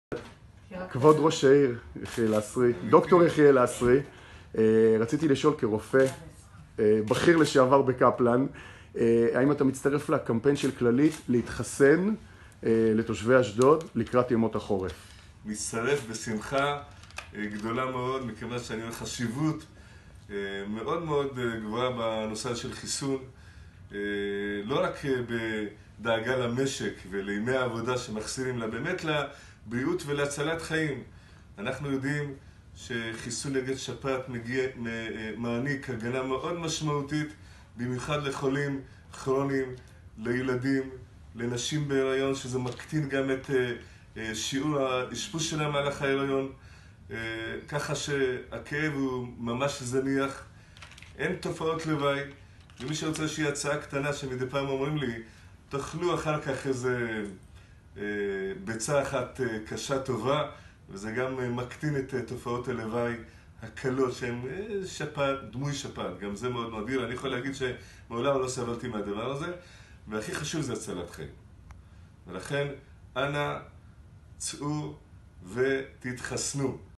מנהלות סיעוד כללית אשדוד מחסנות את ד"ר יחיאל לסרי נגד שפעת. צילום: אופיר לוי, כללית
סרטון מאת Dor